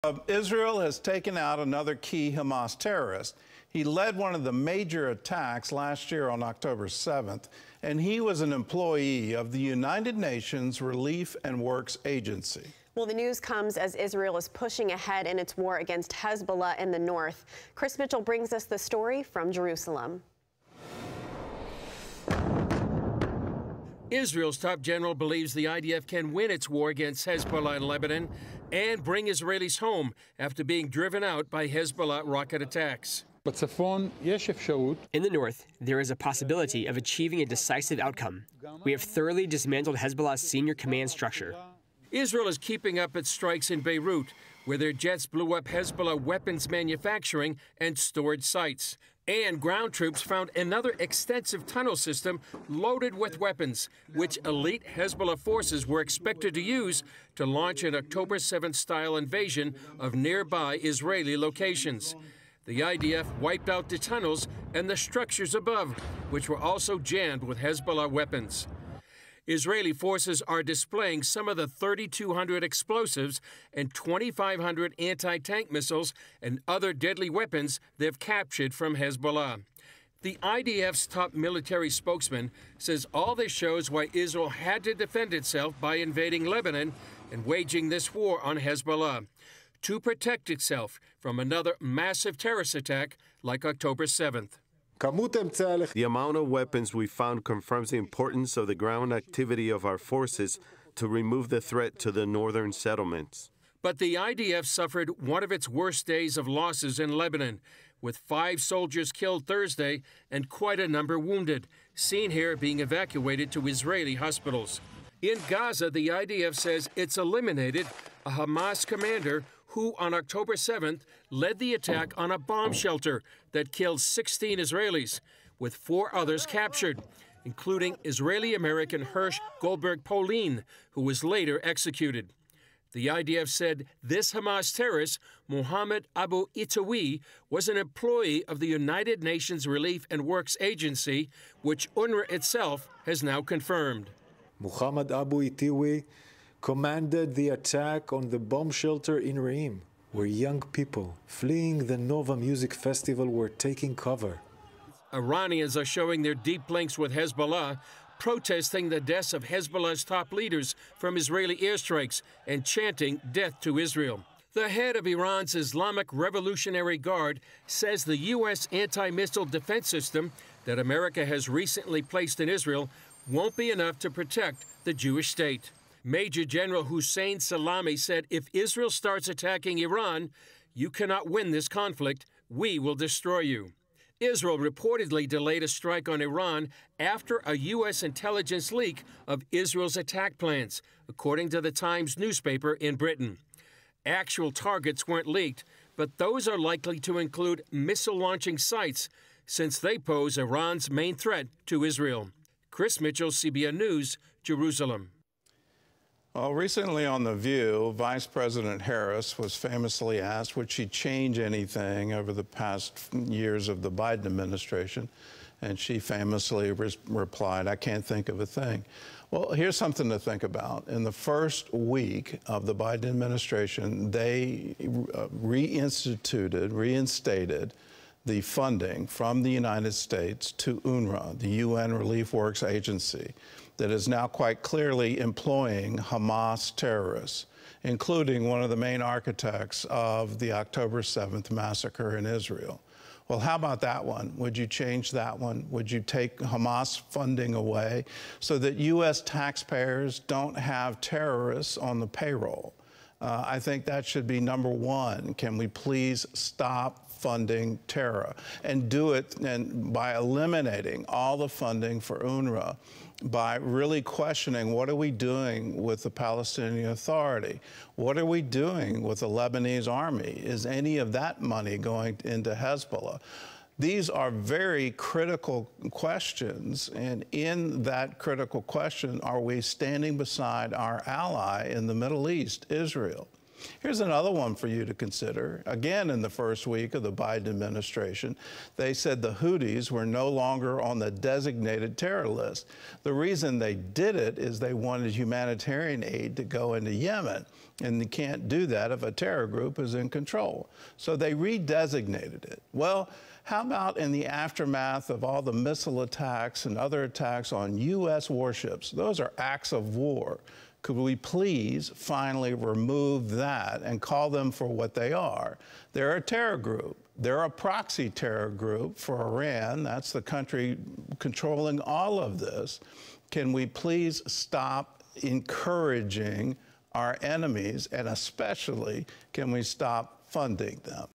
IDF Kills ANOTHER Top Terror Leader
Israel is pressing ahead in its war against Hezbollah as the Iranian proxy continues to launch scores of projectiles at northern Israel daily. Meanwhile, another Hamas commander who was heavily involved on October 7th was eliminated. Israel's top general believes the Israel Defense Forces can win the war they're waging against Hezbollah in Lebanon. IDF Chief of Staff Herzi Halevi declared, "In the north, there is a possibility of achieving a decisive outcome. We have thoroughly dismantled Hezbollah's senior command structure."

CBN's Chris Mitchell has the story.

Want more news from a Christian Perspective? Choose to support CBN